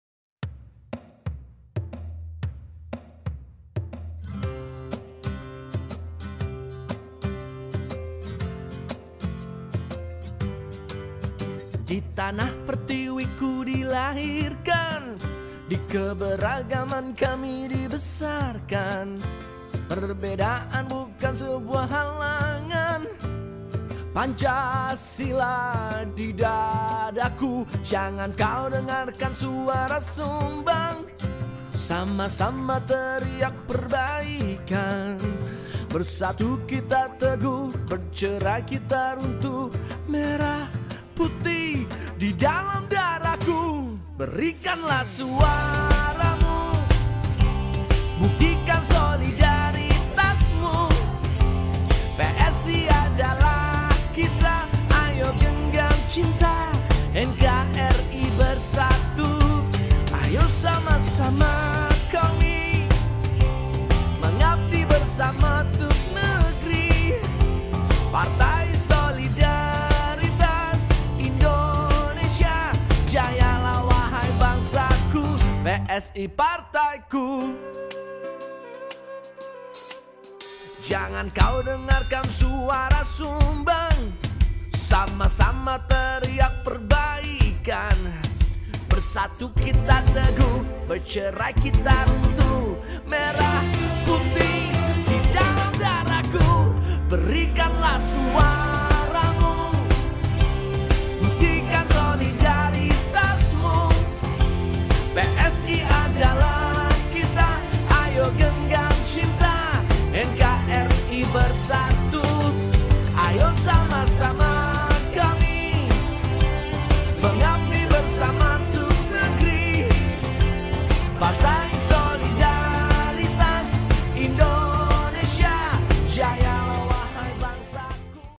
riceboxpsi
Partai Solidaritas Indonesia

Di tengah pandemi dan PPKM berkepanjangan, anak2 muda milenial dari PSI Kota Bogor tetap turun ke jalan berbagi ricebox dengan warga yang belakangan terdampak PPKM.

Dengan tetap mengikuti prokes dalam membagikan ricebox yang dapat langsung disantap oleh orang-orang yang membutuhkan.

Bentuk kepedulian anak - anak muda milenial di Kota Bogor ini menunjukan rasa solidaritas sesama dengan menyisihkan waktu dan tenaga untuk tetap berbagi di tengah pandemi.

Salam solidaritas

Partai Solidaritas Indonesia